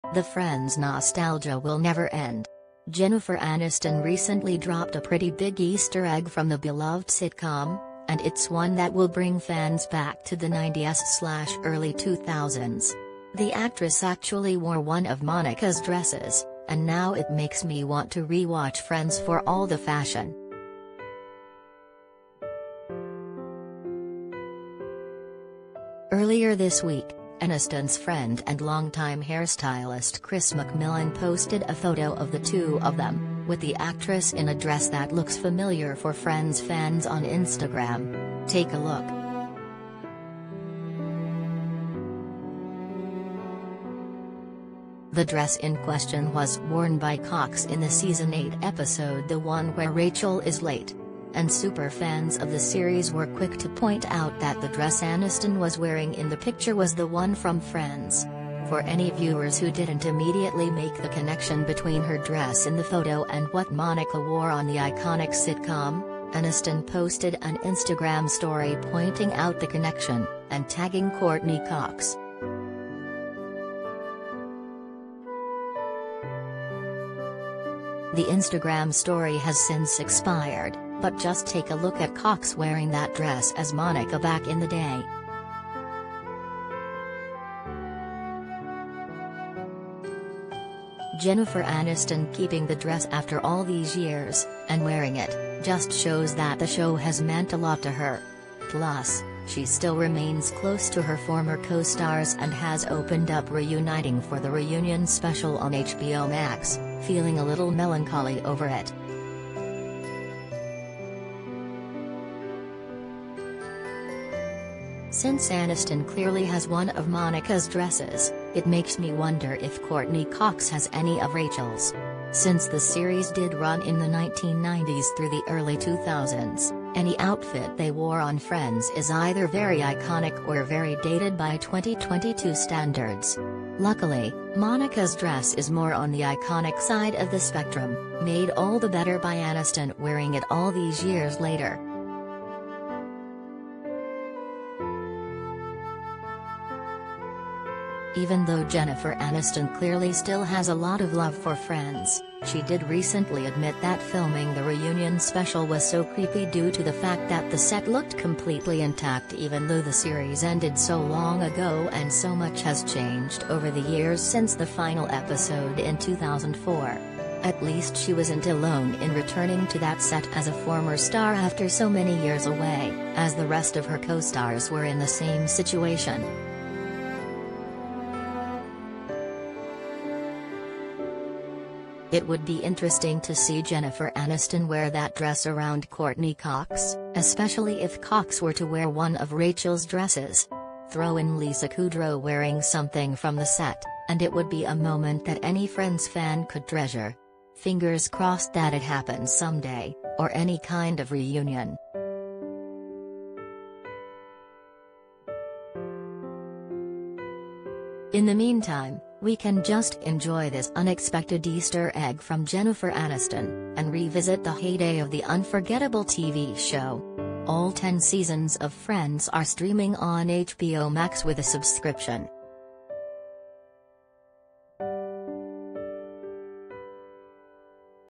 No Big Deal, Just Jennifer Aniston Dropping A Friends Easter Egg For Fans (And Her Pal Courteney Cox
The Friends nostalgia will never end. Jennifer Aniston recently dropped a pretty big Easter egg from the beloved sitcom, and it’s one that will bring fans back to the '90s/early 2000s. The actress actually wore one of Monica’s dresses, and now it makes me want to rewatch Friends for all the fashion.
Earlier this week, Aniston’s friend and longtime hairstylist Chris McMillan posted a photo of the two of them, with the actress in a dress that looks familiar for Friends fans on Instagram. Take a